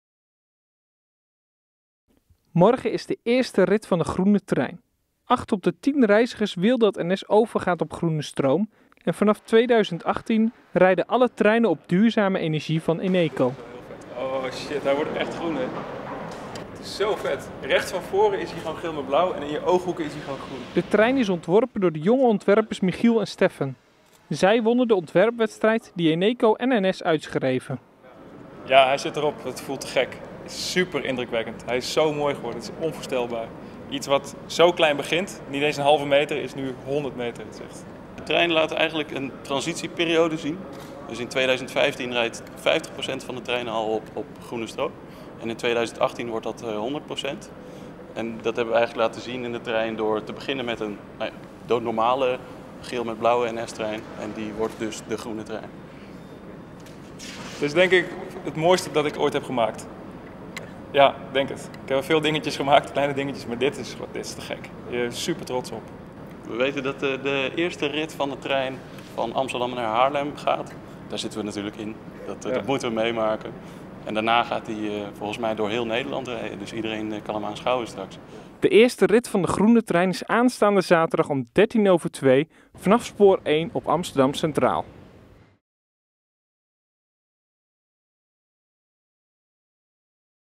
Eerste rit Groene Trein op 175ste verjaardag van het spoor
De Groene Trein beleeft op zaterdag 20 september - de 175ste verjaardag van het spoor - zijn eerste rit. De trein is ontworpen door de jonge Amsterdamse  ontwerpers Michiel van Sinderen en Steffen de Jonge. Zij wonnen de ontwerpwedstrijd die Eneco en NS uitschreven om te laten zien dat alle elektrische treinen op nieuwe groene stroom gaan rijden. Gisteren kregen de winnaars voor het eerst “hun” trein te zien.

Ontwerp
 De preview in de werkplaats van NedTrain was een spannend moment. Terwijl medewerkers bezig waren om de transformatie van blauw-gele dubbeldekker naar Groene Trein af te ronden, konden de ontwerpers voor het eerst zien hoe hun ontwerp er op een levensgrote trein uitziet. De reizigers krijgen straks een trein te zien waarbij de bekende gele en blauwe opgaan in duizenden blokjes. Zo geven ze de illusie dat de trein groen is, zonder dat de kleur groen daadwerkelijk op de trein voorkomt. Of ze in deze opzet zijn geslaagd, kan de reiziger zelf beoordelen. Na de eerste rit aanstaande zaterdag, gaat de Groene Trein kris-kras door Nederland rijden.

Groene stroom
In totaal deden 72 jonge ontwerpers mee met de ontwerpwedstrijd. Uiteindelijk werden Michel en Steffen gekozen uit een jury bestaande uit onder meer Daan Roosegaarde, Jelle Brandt Corstius, Hans Ubbink, Erik Kessels en Arriën Kruyt. De Groene Trein staat symbool voor de toekomst, waarin het voor de reiziger mogelijk wordt om schoon te reizen. In 2015 rijden de treinen voor 50% op groene stroom die met nieuwe windmolenprojecten wordt opgewekt. Vanaf 2018 rijden alle elektrische treinen volledig op windenergie. Dat zijn Eneco en de gezamenlijke spoorpartijen dit voorjaar overeengekomen.